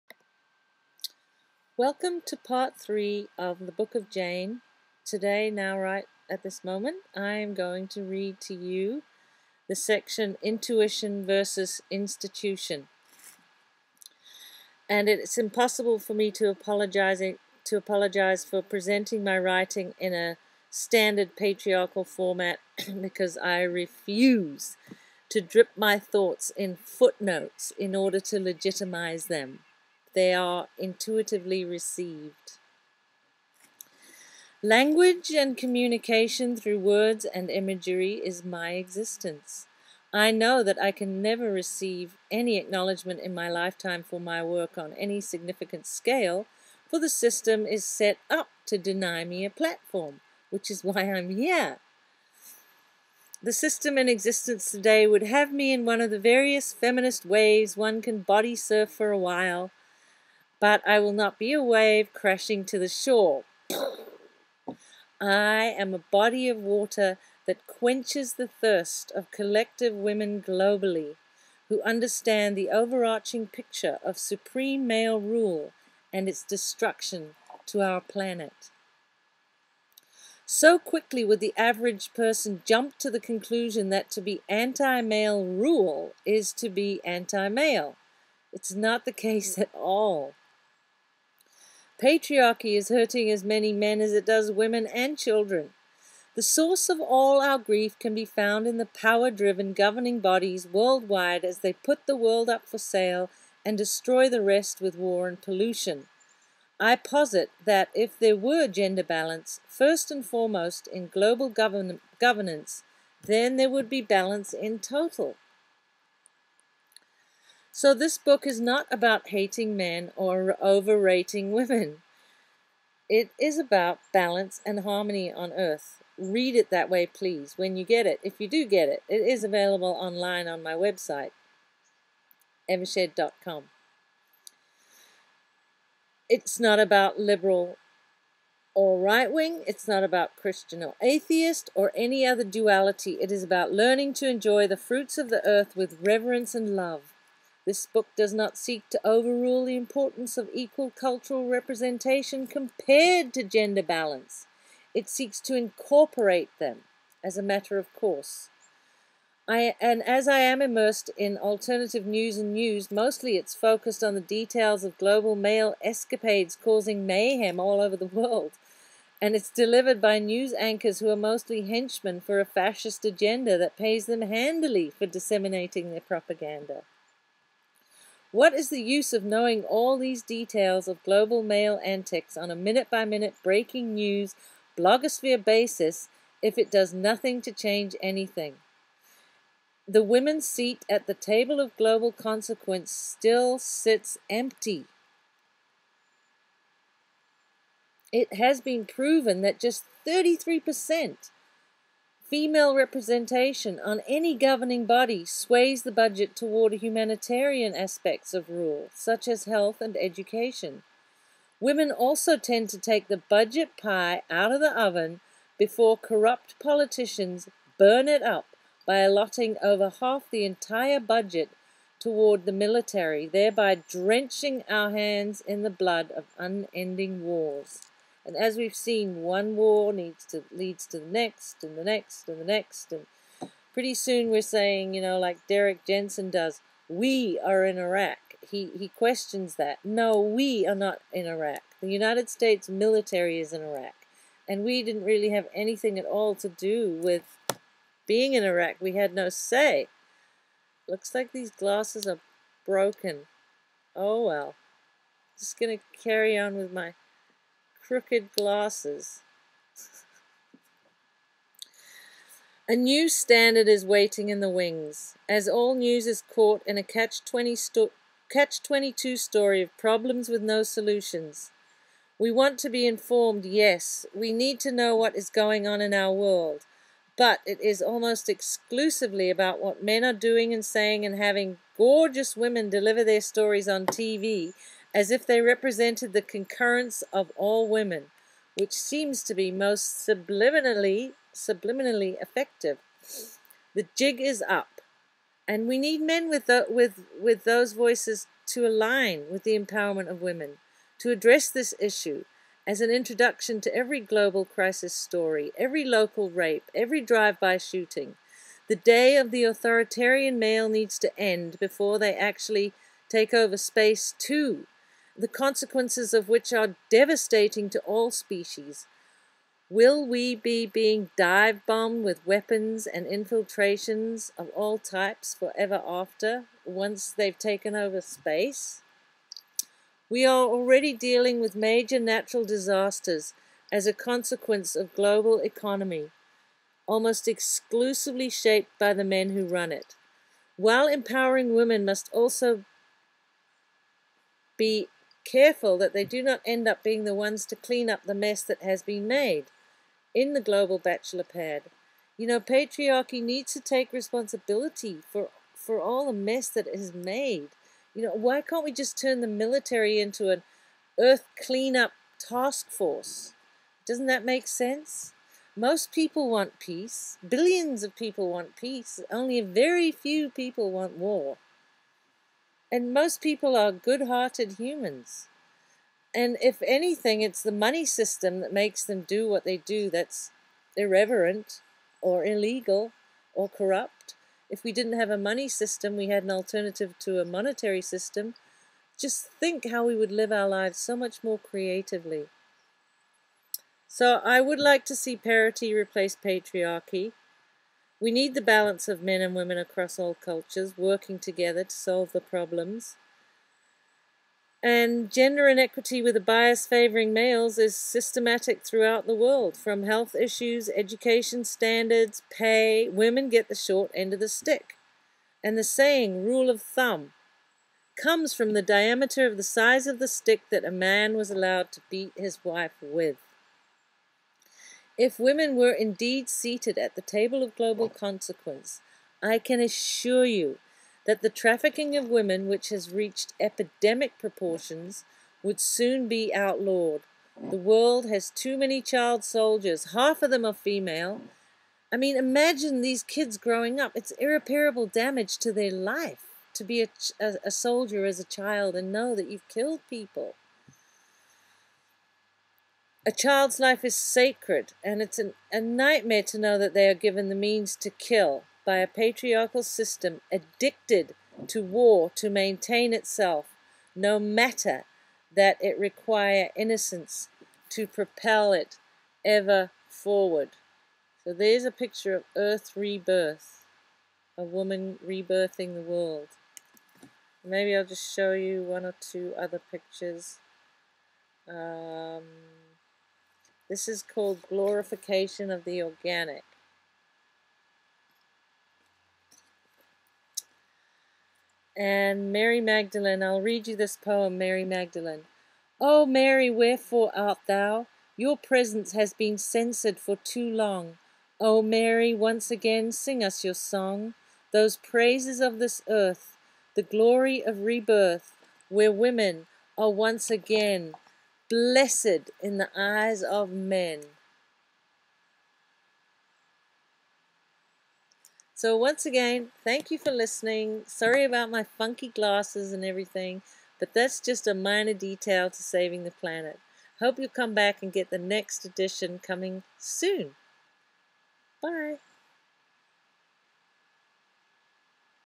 The Book of Jane 5
versus Institution...

It is impossible for me to apologize for not presenting my writing in a  standard  patriarchal format; I feel no  reason to drip my  thoughts  in footnotes in order to legitimize them.
  Language and communication through words and imagery is my existence. I know that I can never receive any acknowledgement in my lifetime for my work on any significant scale for the system is set up to deny me a platform. The system in existence today would have me in one of the various feminist waves one can bodysurf for a while. I will not be a wave crashing to the shore. I am a body of water that quenches the thirst of collective woman globally who understand the over arching picture of supreme male rule and its destruction to our planet.
  So quickly would the average reader jump to the conclusion that to be anti-male rule is to be anti male! Not the case at all. Patriarchy is hurting as many men as it does women and children. The source of all our grief can be found in the power driven corporations kidnapping governments  to legitimize their bidding as they put the world up for sale and destroy the rest with war and pollution. I posit that if there were gender balance, first and foremost in global governance and  business then there would be balance in total.
  My premise is not about hating men, or over rating women, it is about balance and harmony. It is not about liberal or right wing, Christian or atheist, or any other
duality, it is about learning to enjoy the fruits of earth with reverence and love.  My work does not seek to deny the importance of equal cultural representation compared  to gender balance either, it seeks to incorporate them as a matter of course. I am immersed in alternative news and views. Mostly it is focused on the details of global male escapades causing mayhem, and it is delivered by news anchors who are mostly henchmen for a fascist agenda  that pays them handily for disseminating the propaganda for a profiteering war agenda.
   What is the use of knowing all these details of global male antics on a minute-by-minute, breaking news/blogosphere basis if it does nothing to change anything? The women's seat at the table of global consequence still sits empty. It has been proven that just 33% female representation on any governing body sways the budget toward the more humanitarian aspects of rule such as health and education. Women also tend to take the budget pie out of the oven before corrupt politicians burn it up by allotting over half the entire budget  toward the military thereby drenching our hands in the blood of unending war.
   A new standard is waiting in the wings, as all news is caught in a catch 22 story of problems with no solutions. We want to be informed, yes, we need to know what is going on in our world but it is almost exclusively about what men are doing and saying and having gorgeous women deliver their stories on TV as if they represented the concurrence of all women, (which seems to be most subliminally effective).